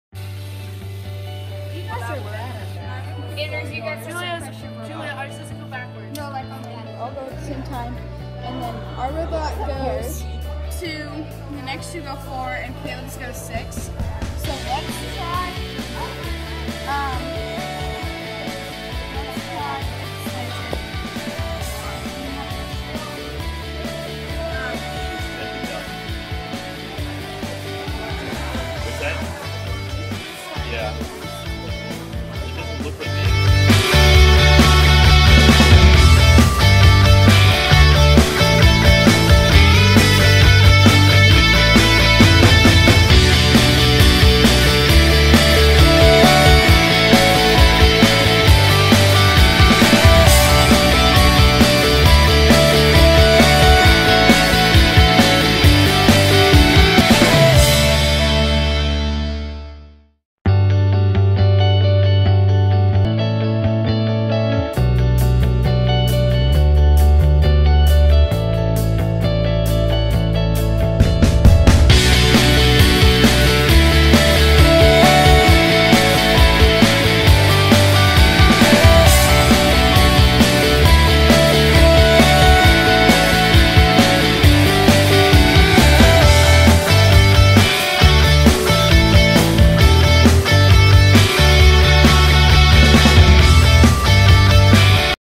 Wave1StudentFilm
I had my students document their progress and make a film about programming the Wave.